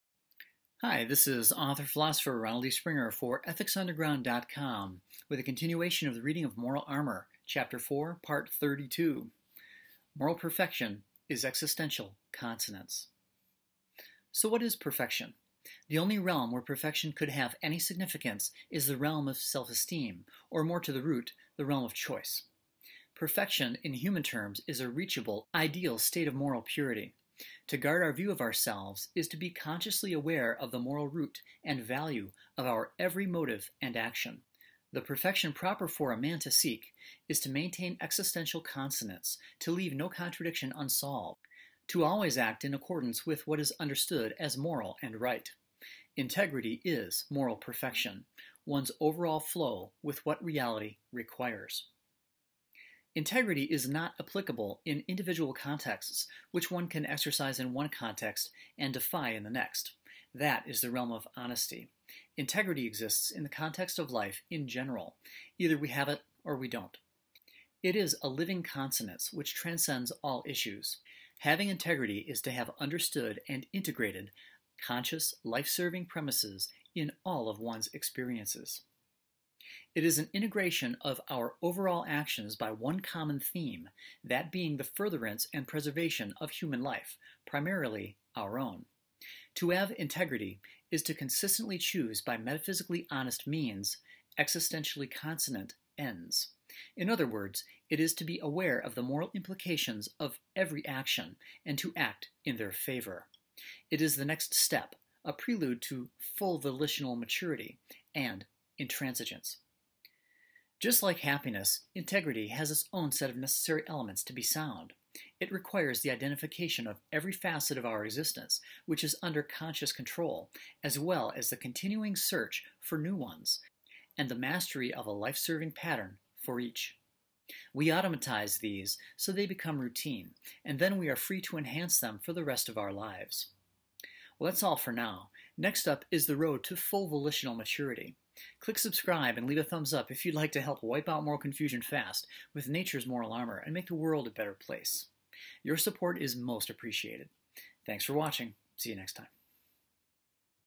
Self-Made Man: Moral Perfection | Reading of Moral Armor CH4P32
Moral Perfection is Existential Consonance. So what is perfection? The only realm where perfection could have any significance is the realm of self-esteem, or more to the root, the realm of choice.

"The Care and Feeding of Freedom" eBook is included w/any $5 pledge!

Perfection in human terms is a reachable, ideal state of moral purity. To guard our view of ourselves is to be consciously aware of the moral root and value of our every motive and action. The perfection proper for a man to seek is to maintain existential consonance: to leave no contradiction unsolved, to always act in accordance with what is understood as moral and right. Integrity is moral perfection—one’s overall flow with what reality requires.

Integrity is not applicable in individual contexts, which one can exercise in one context and defy in the next; that is the realm of honesty. Integrity exists in the context of life in general; either we have it or we don’t. It is a living consonance, which transcends all issues. Having integrity is to have understood and integrated conscious life-serving premises in all of one’s experiences. It is an integration of our overall actions by one common theme, that being the furtherance and preservation of human life, primarily our own. To have integrity is to consistently choose by metaphysically honest means, existentially consonant ends. In other words, it is to be aware of the moral implications of every action, and to act in their favor. It is the next step; a prelude to full volitional maturity and intransigence.

Just like happiness, integrity has its own set of necessary elements to be sound. It requires the identification of every facet of our existence which is under conscious control, as well as the continuous search for new ones, and the mastery of a life-serving pattern for each. We automatize these so they become routine, and then we are free to enhance them for the rest of our lives.

Moral Armor on Amazon

The Care and Feeding of Freedom on Amazon

Please visit EthicsUnderground.com to learn more about the philosophy of Moral Armor.